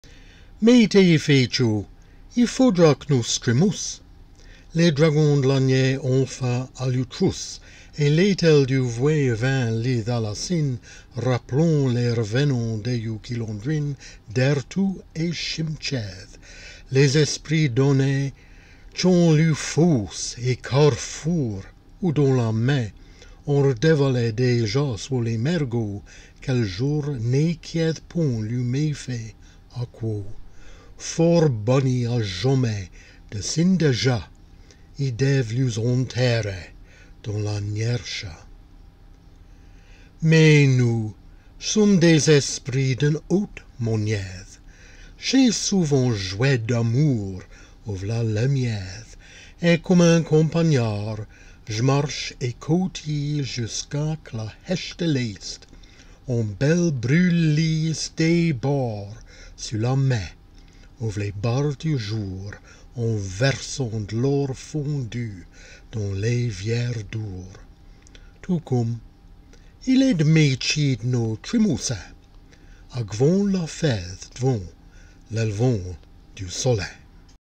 Un rêve à la Saint Jean
A Midsummer Night's Dream in Jèrriais
(A partial reading of Act 3, scene 2, from the beginning as far as "what fools these mortals be")